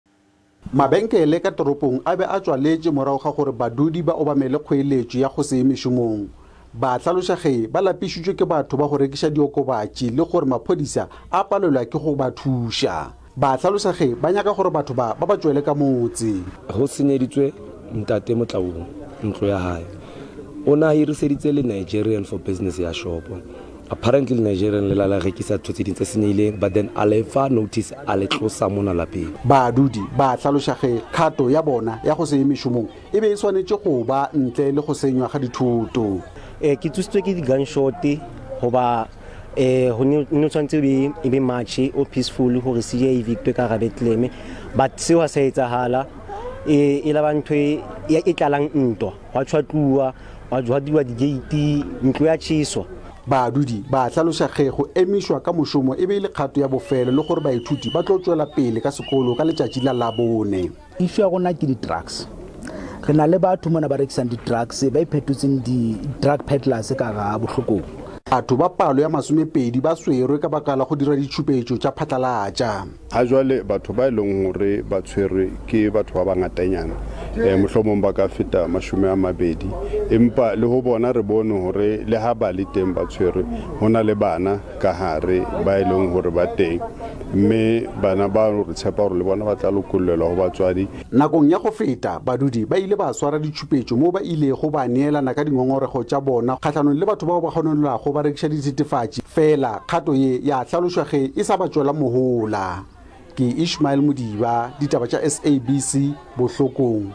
Baagi ba lela ka gore bagwebi ba rekisetsa bana ba bona diritibatsi
Baagi ba ba tshakgetseng ba motse setoropo wa Bohlokong kwa Bethlehem, ba tsogile ba jele ntlo e mmelaelwa wa go gweba ka diritabatsi a neng a nna mo go yona. Se ka nako eo baagi ba neng ba bolotsa ditshupetso kgatlhanong le bagwebi ba diritibatsi mo kgaolong ya bona.

For more news, visit sabcnews.com and also SABCNews SABCDikgang on Social Media.